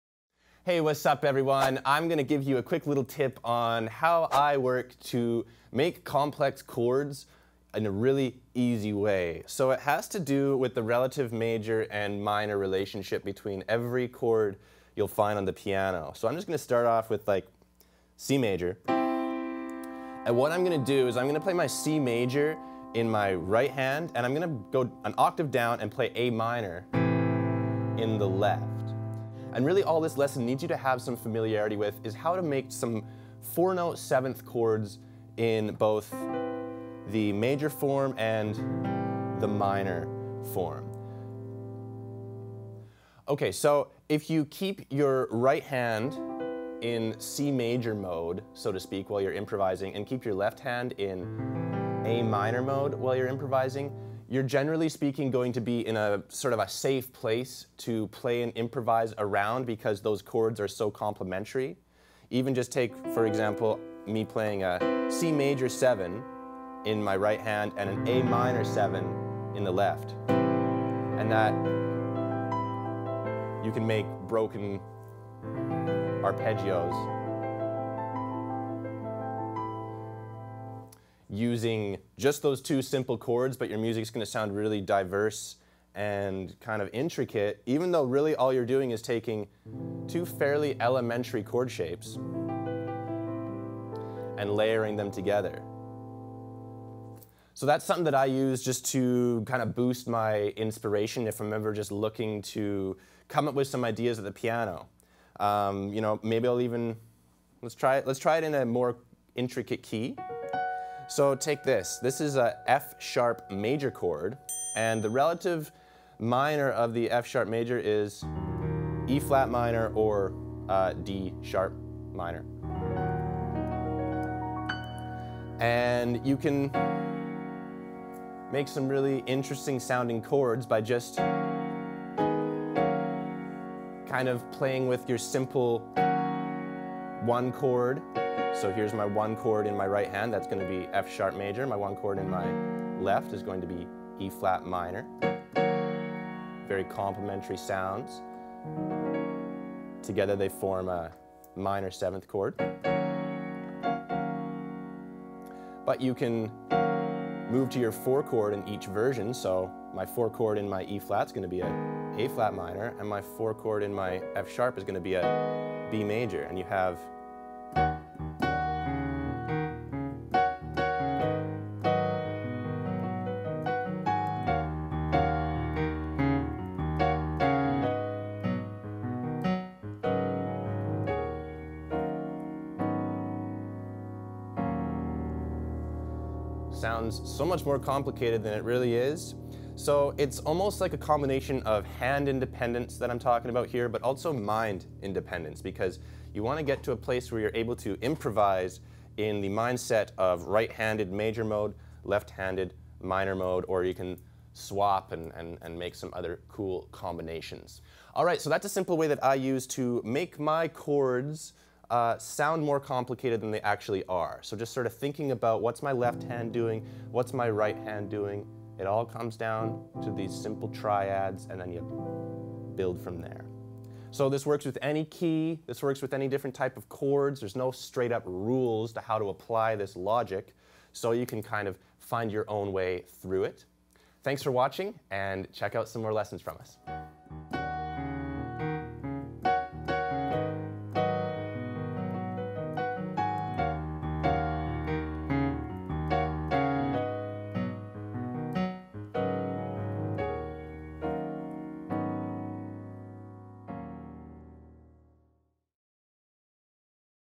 How To Build Complex Piano Chords
One of the questions I get asked a lot is how to play complex chords in a way that is easy, intuitive and immediate. I totally understand why people ask this question because I still remember the days when I’d look at complex chord shapes and be totally mystified at what was going on!

The good news is if you’ve spent time practicing your triads and basic major and minor chords, you have a really solid foundation that you need to jump into the next step: building major and minor 7th chords.

The best way to do this is by building on a little something I like to call ‘Focus Independence.’